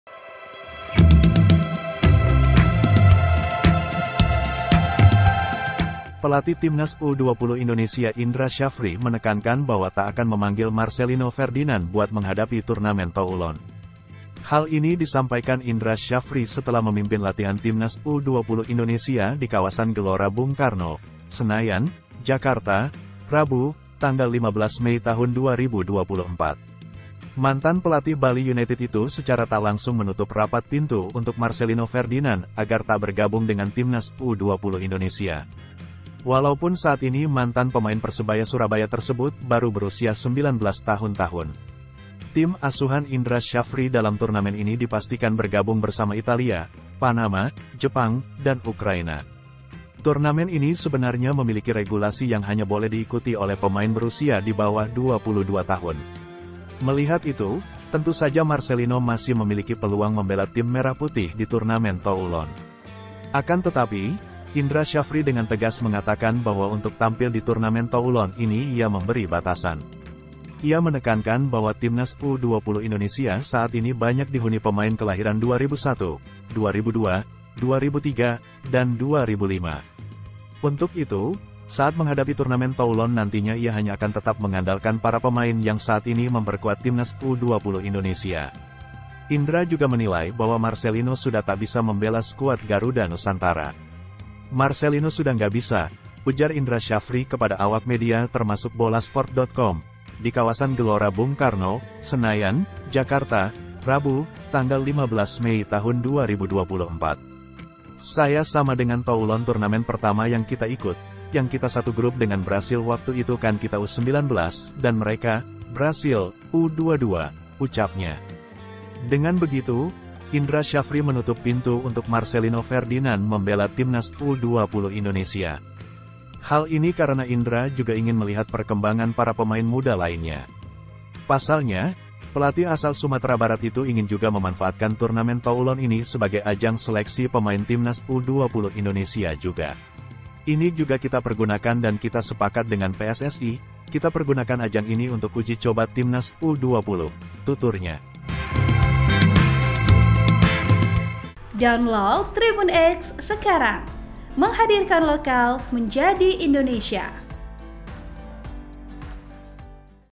Timnas U20 Ikut Ajang Bergengsi, Indra Sjafri Tegas Tutup Rapat Pintu Untuk Marselino Ferdinan
Pelatih Timnas U-20 Indonesia Indra Sjafri menekankan bahwa tak akan memanggil Marselino Ferdinan buat menghadapi Turnamen Toulon.
Hal ini disampaikan Indra Sjafri setelah memimpin latihan timnas U-20 Indonesia di Kawasan Gelora Bung Karno, Senayan, Jakarta, Rabu (15/5/2024).

(HDK)